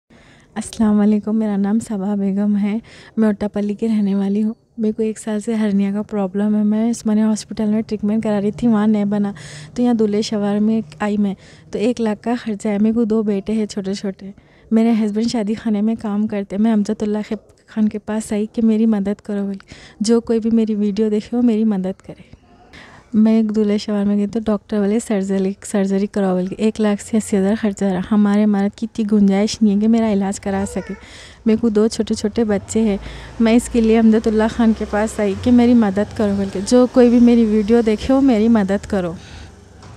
Help Saba Begum suffering from Hernia for her surgery costing Rs/ 1.0 lakh:
This is to state that during my Praja_Darbar at Darul_Aman Chanchalguda one Saba Begum resident of H No: 19-3-286/A/D, Vattepally approached me for financial help for her surgery who is suffering from hernia.

First her treatment is undergoing in Osmania General Hospital but because of ill health she has shifted to Princess Durreshewar Hospital, Purani Haveli where doctor advice her for surgery. Her husband is a daily wage laborer has to look after two son's by which she unable to arrange amount for her surgery amount. Kindly help her by donating some amount in her bank account or Google-Pay.

Saba Begum
State Bank of India, Vattepally

Thanking You
Yours Sincerely

(Spokesman) MBT